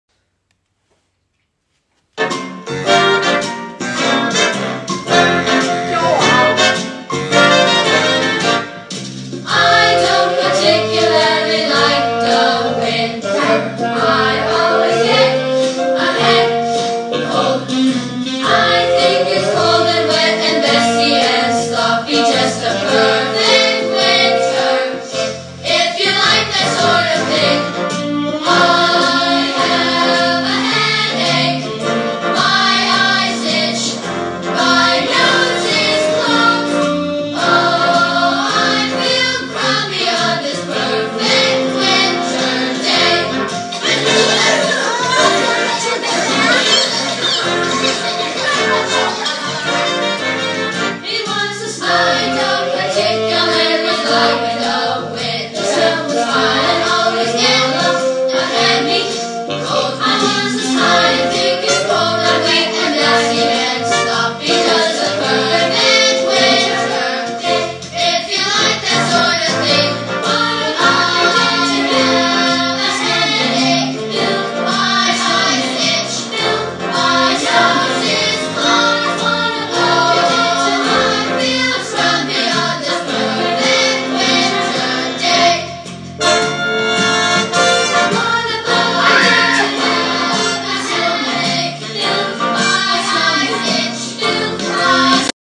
Morning Dance
He is teaching me about his school Xmes performance dance show . Hahahahaha, I am try to catch up with him.